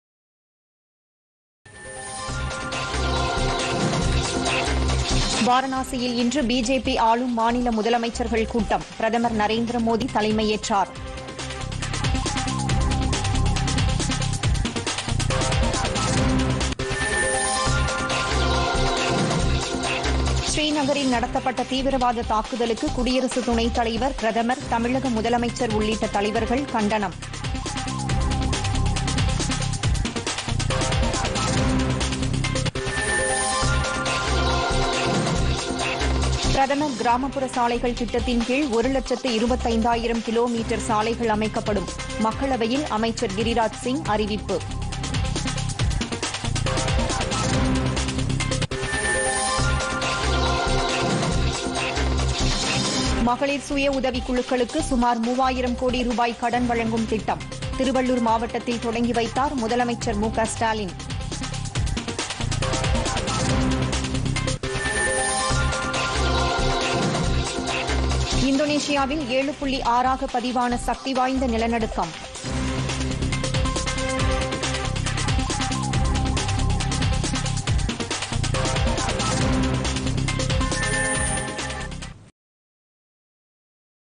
பொதிகை பிற்பகல் 2.00 மணி தலைப்புச் செய்திகள் [14.12.2021]
1)  வாரணாசியில் இன்று பிஜேபி ஆளும் மாநில முதலமைச்சர்கள் கூட்டம் - பிரதமர் நரேந்திர மோதி தலைமை ஏற்றார்.

2)  ஸ்ரீநகரில் நடத்தப்பட்ட தீவிரவாத தாக்குதலுக்கு குடியரசுத் துணைத் தலைவர், பிரதமர், தமிழக முதலமைச்சர் உள்ளிட்ட தலைவர்கள் கண்டனம்

3)  பிரதமர் கிராமப்புற சாலைகள் திட்டத்தின் கீழ்  ஒரு லட்சத்து 25 ஆயிரம் கிலோ மீட்டர் சாலைகள் அமைக்கப்படும் - மக்களவையில் அமைச்சர் கிரிராஜ் சிங் அறிவிப்பு.

4) மகளிர் சுய உதவிக் குழுக்களுக்கு சுமார் மூவாயிரம் கோடி ரூபாய் கடன் வழங்கும் திட்டம் :
   திருவள்ளூர் மாவட்டத்தில் தொடங்கி வைத்தார் முதலமைச்சர் மு க ஸ்டாலின்

5)  இந்தோனேஷியாவில் 7 புள்ளி 6ஆக பதிவான சக்தி வாய்ந்த நிலநடுக்கம் .